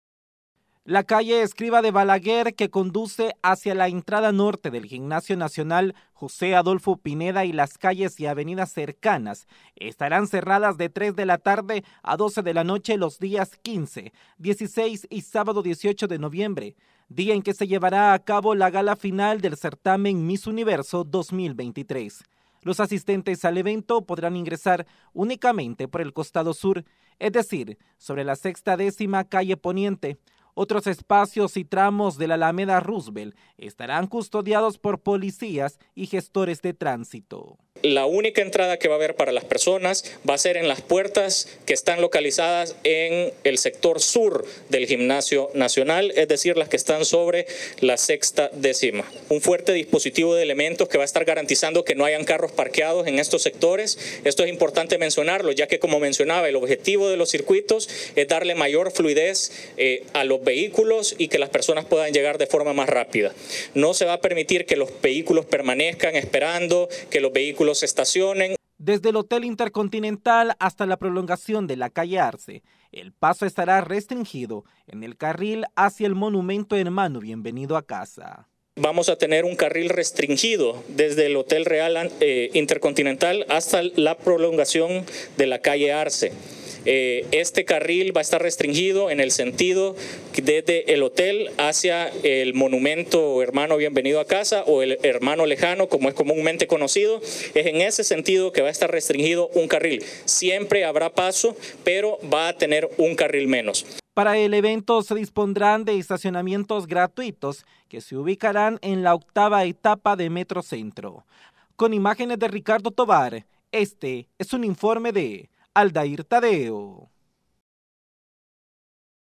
Cierre de calles por actividades Miss Universo 2023
Calles y avenidas de San Salvador serán cerradas a conductores debido a las actividades más importantes del certamen de belleza miss universo, conozca cuáles son esas arterias.